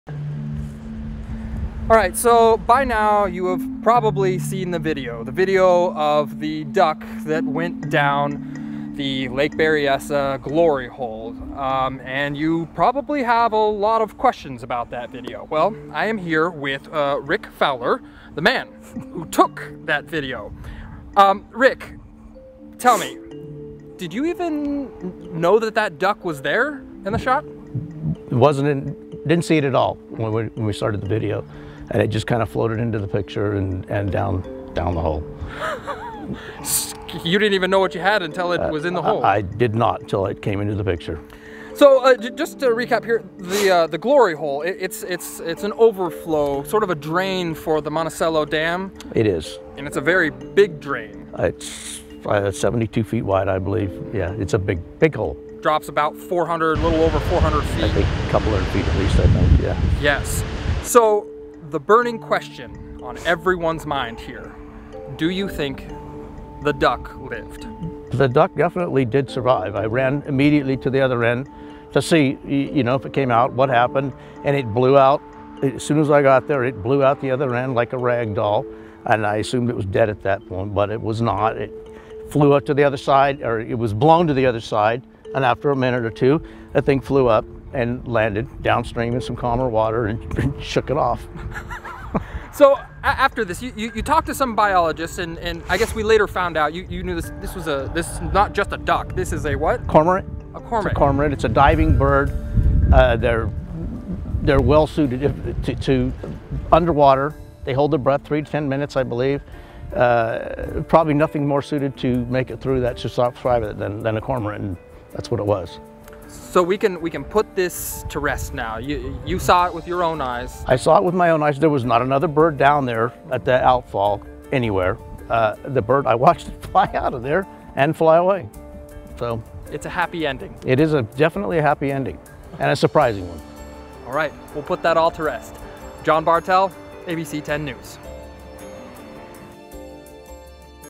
Rick Fowler, the man who captured video of the ‘Glory Hole Duck,’ explains how the bird survived
We've all been hearing a lot about it: A duck fell into Lake Berryessa's spillway, also known as the Glory Hole. The whole thing was caught on camera. The man who recorded the video, Rick Fowler, says the duck survived, but ABC10’s John Bartell had some follow up questions.